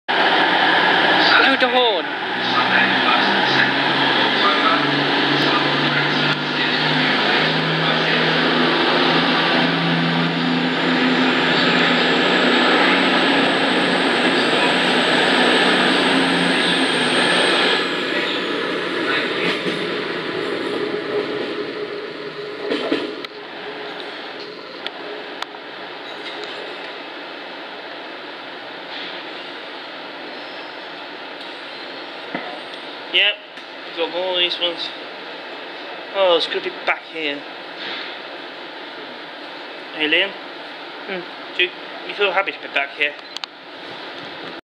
crss country service departig from basingstoke going to manchester picadilly
Basingstoke train station is a good location for
south west trains go there on great cross country
way service goes there great western service
trained I called gwr